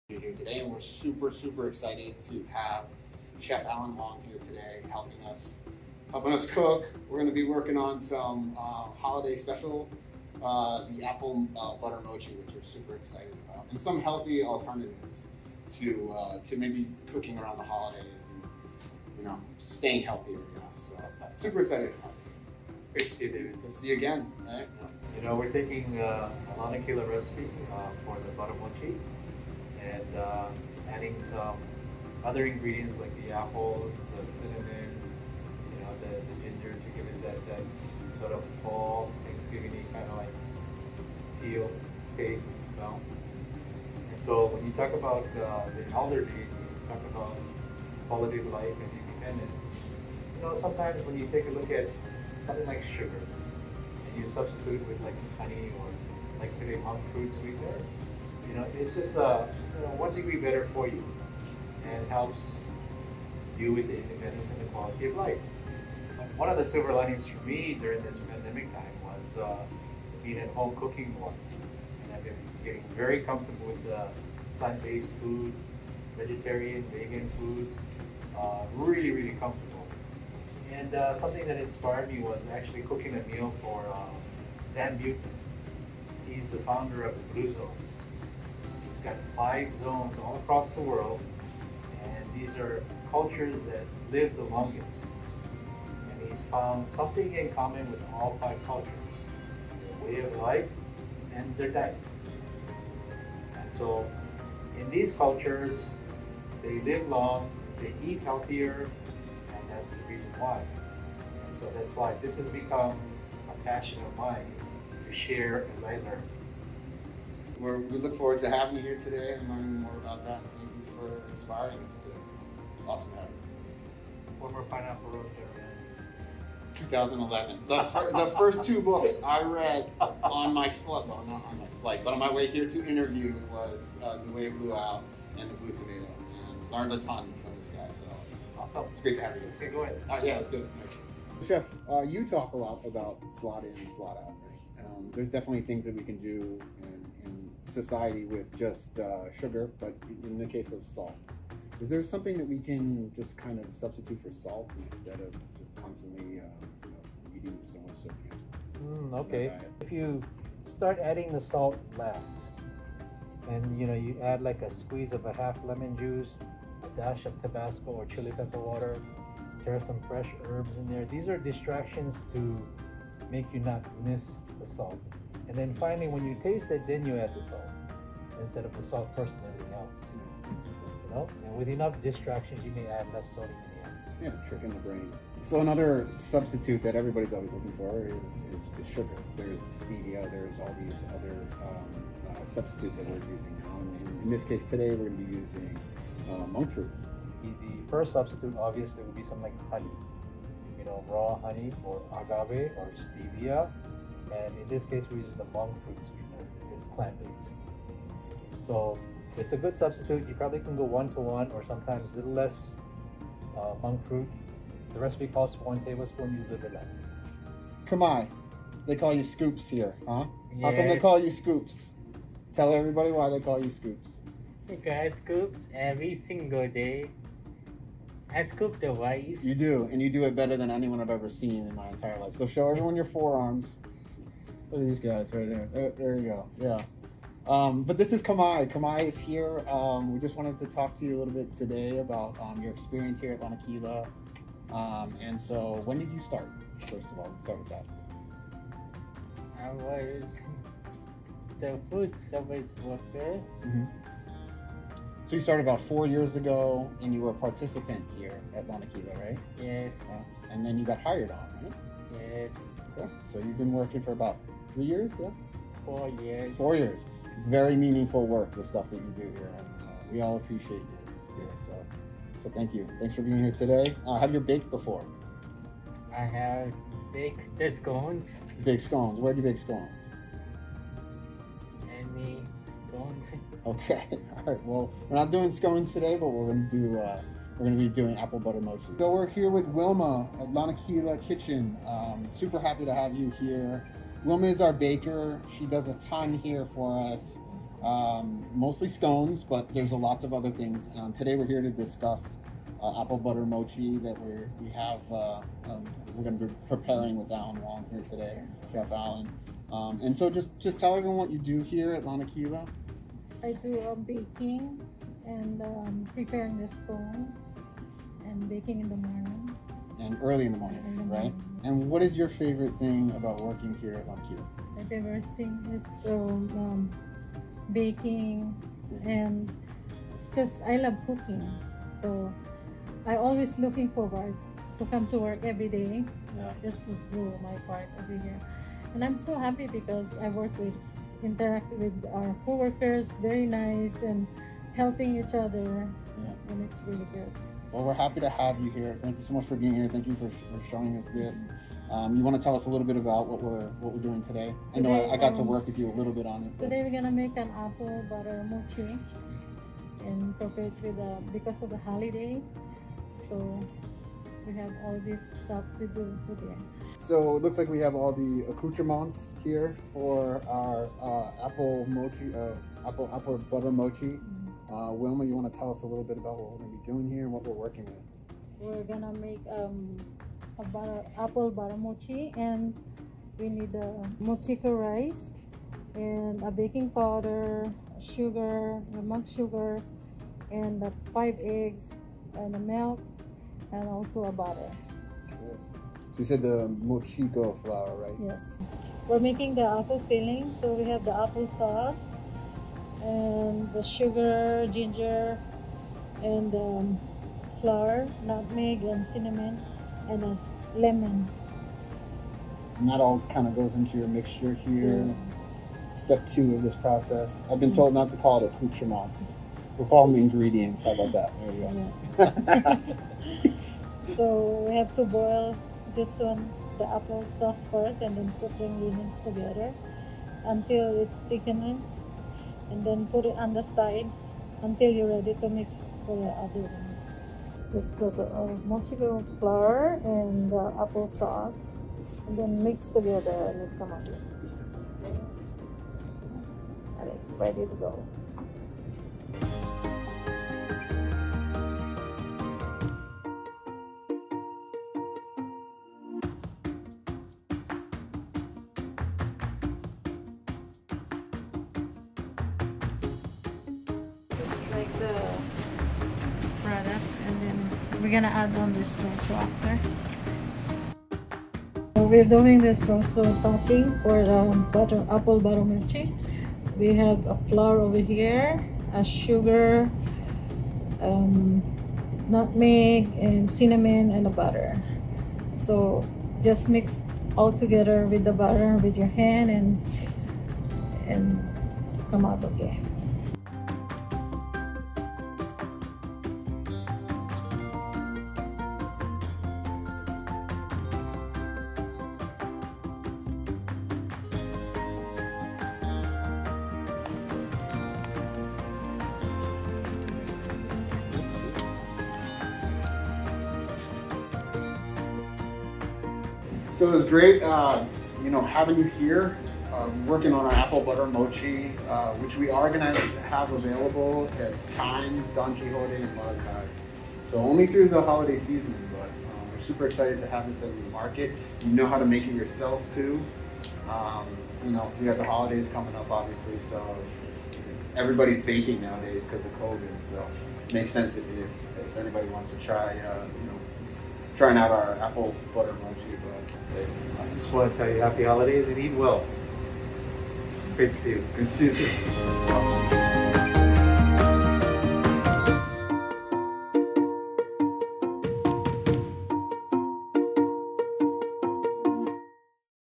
Apple Butter Mochi with Chef Alan Wong at Lanakila Kitchen
In a special collaboration with world-renowned Chef Alan Wong, Lanakila Kitchen introduces its new Apple Butter Mochi this holiday season. As a proponent of healthier living, Chef Alan inspired Lanakila Kitchen to innovate one of its baked goods by incorporating healthier ingredients like monk fruit sweetener and the superfood ginger. This sweet treat is available now for a limited time only at Don Quijote, Marukai and Times Supermarket locations.